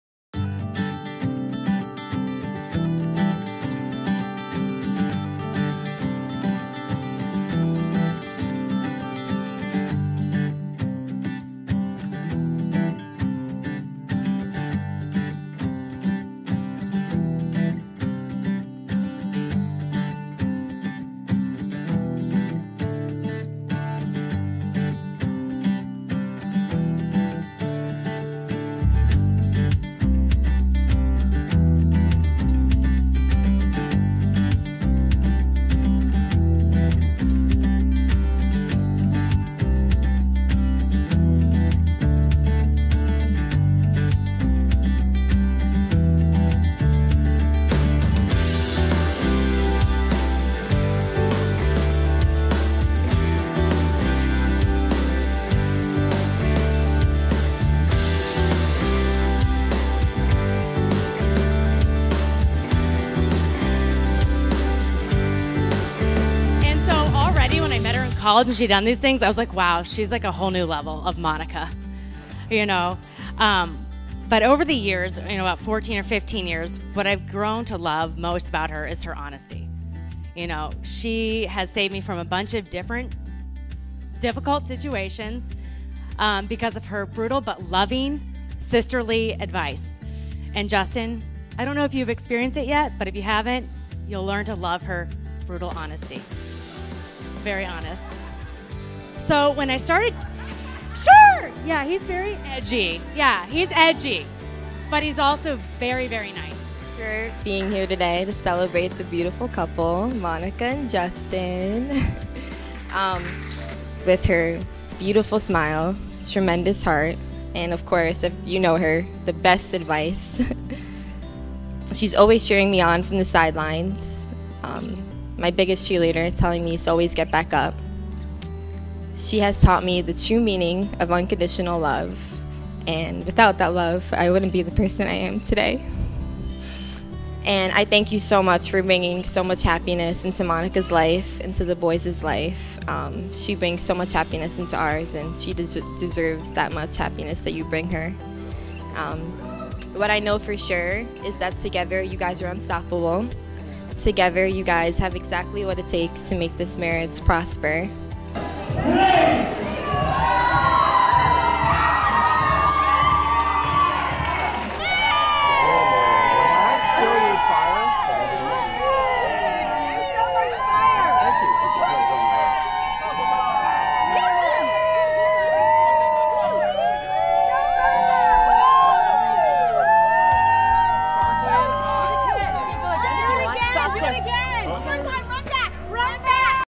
St Augustine Casa Monica Wedding Highlight Video
Love Story Highlight of Monica + Justin 8-15-20
Wedding Ceremony in the Flagler Ballroom of Saint Augustine historic Casa Monica Resort, Autograph Collection and reception held in the Grand Ballroom | Wedding Video Trailer | Favorite Studios Photography Cinematography | Jerry and Karen Favorite, Photographers + Videographers